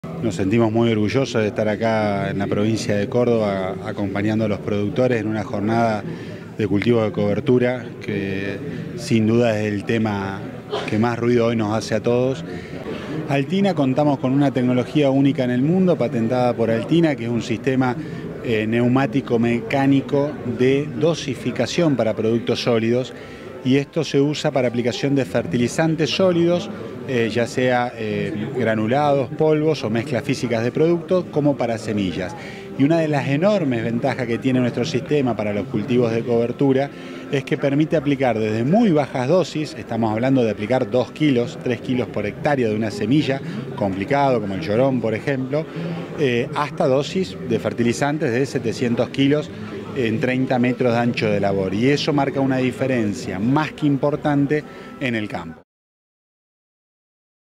Altina presente en la Primera Jornada de Cultivos de Cobertura
Participamos de la primera “Jornada de Cultivos de Cobertura de la Región Centro de Córdoba”. Informar, pensar y planificar, para la campaña 2020